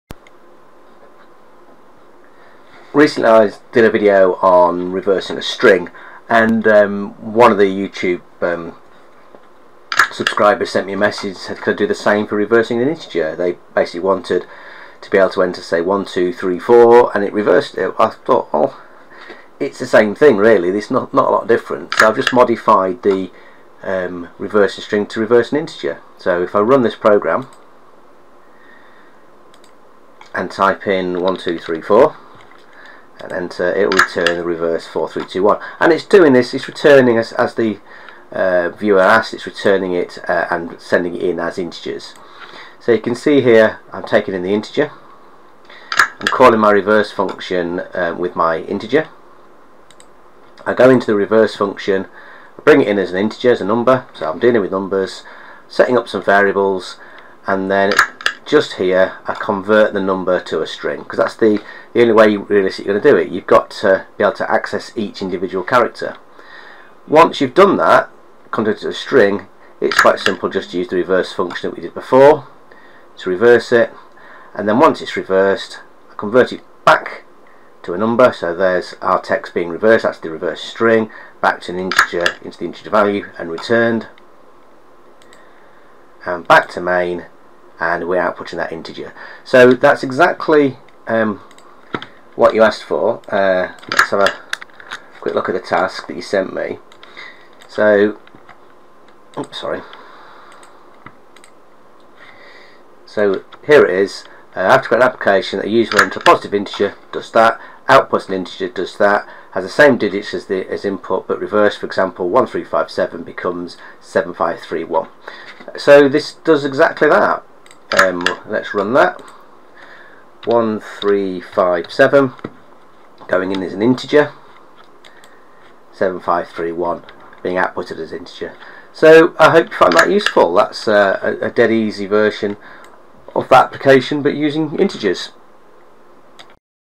Flowgorithm - Reverse and Integer
How to reverse a integer value.  eg 1234 becomes 4321